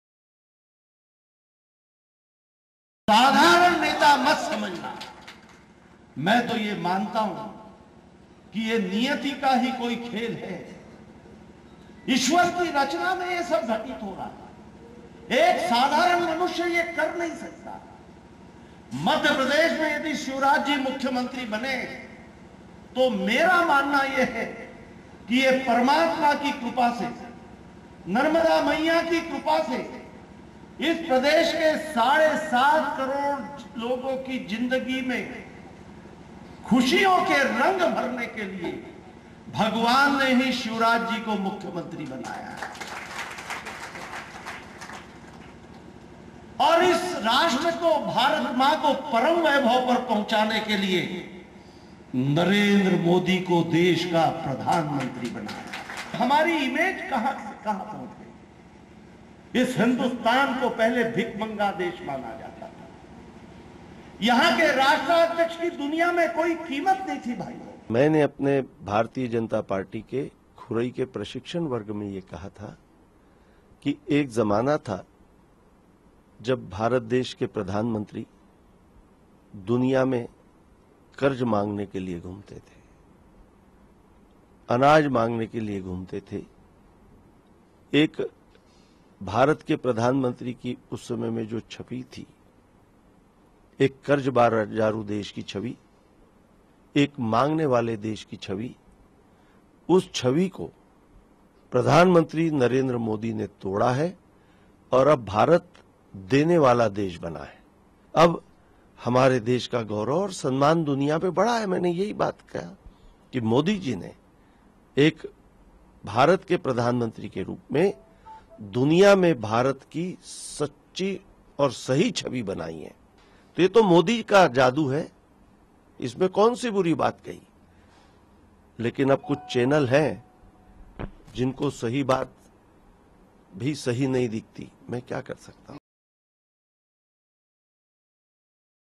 BJP leader says India was a beggar state before PM Modi, stands firm on remark
Bhopal/ Sagar, Oct 04 (ANI): Madhya Pradesh BJP unit President Nandkumar Singh Chauhan said that India was a beggar state before Prime Minister Narendra Modi came to power. He said that India used to ask for funds and help earlier from other nations, but the image of India has changed drastically after PM Modi took oath as the prime minister of the country. Chauhan remained firm on his remark and said that there is nothing wrong in what he had said. He maintained that the prime minister has given a new status to India which was not recognized on the international forum earlier.